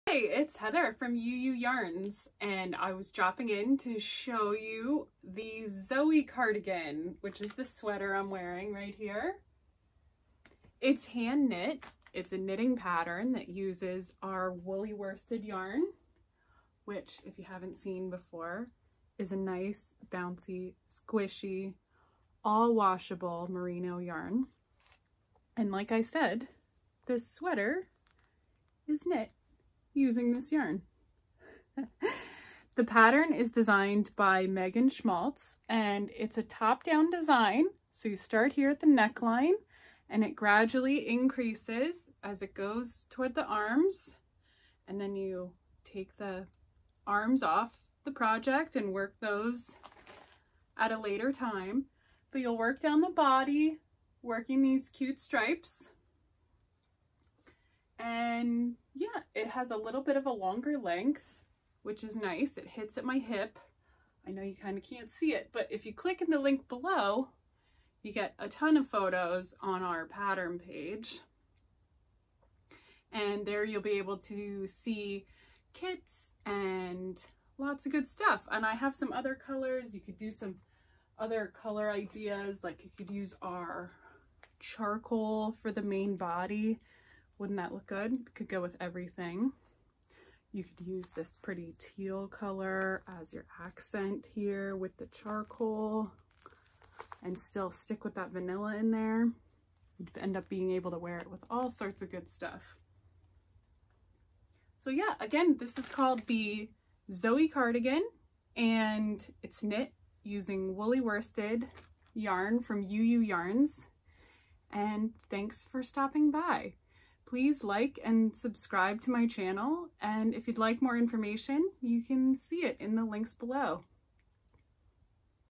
Zoey Cardigan top-down raglan knitting pattern from Ewe Ewe Yarns
The Zoey Cardigan is designed by Meaghan Schmaltz to be a seamless knitting project. Using Ewe Ewe Wooly Worsted merino wool yarn, this project is fun to knit, easy to care for and even better to wear!